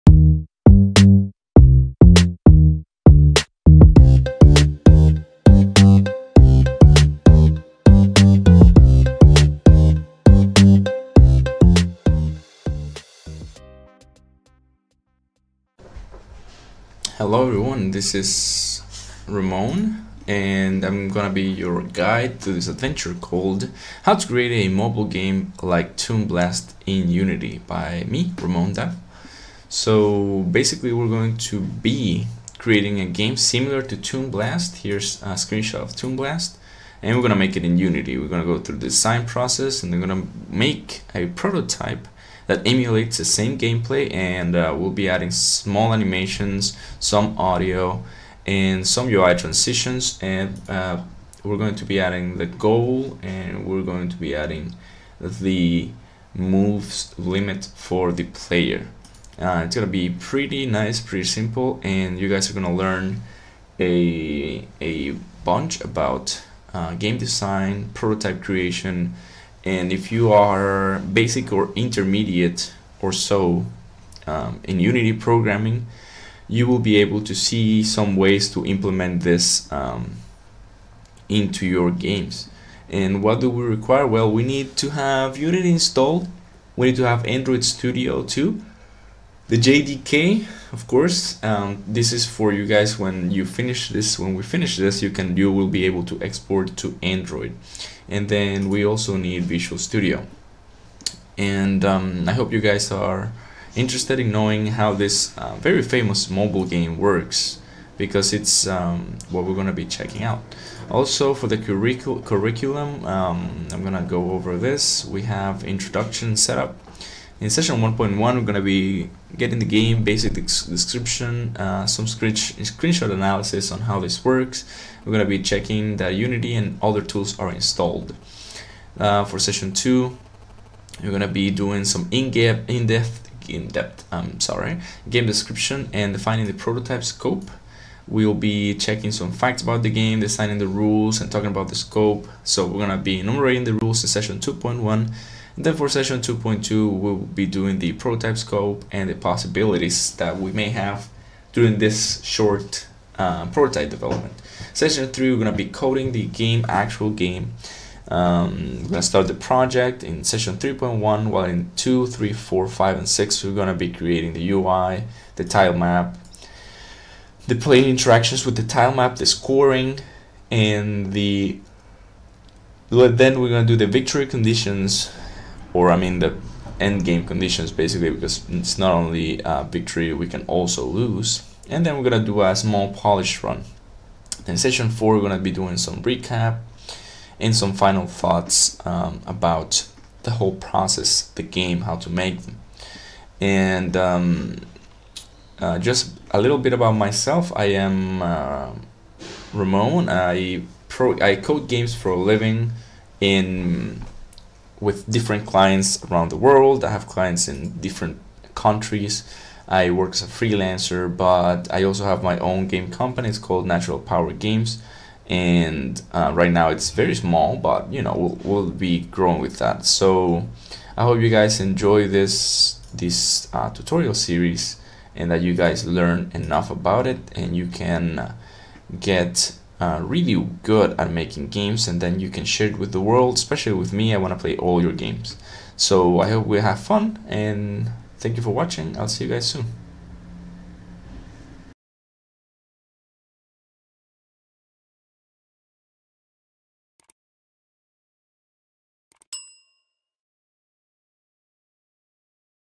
How To Create A Mobile Game Like Toon Blast In Unity | Introduction
In this Unity tutorial, you will learn to create a mobile game like Toon Blast in Unity
During this tutorial, We'll go through the game design process, then make a prototype that emulates the same gameplay. We'll also add some animations, audio, and UI transitions. Any questions or assistance required will be answered on the spot.

Session 01: Introduction and Setup

 Session 02: Game basic description, screenshot analysis

 Session 03: Checking Unity and other tools are installed

Session 04: In-depth Game Description and Defining the prototype's scope.

Session 05: Rules enumeration

Session 06: Prototype Scope and Possibilities

Session 07: Coding the game.

 Session 08: Starting the Unity Project and choosing assets from the asset store. Also generating some sounds to use later.

 Session 09: Creating the UI and applying the transitions between them

 Session 10: Creating the tilemap and filling it up with tiles.

 Session 11: Player interactions with tilemap, scoring.

 Session 12: Main mechanic.

Session 13: Bug fixes, final polish run.

Session 14: Exporting to APK, Recap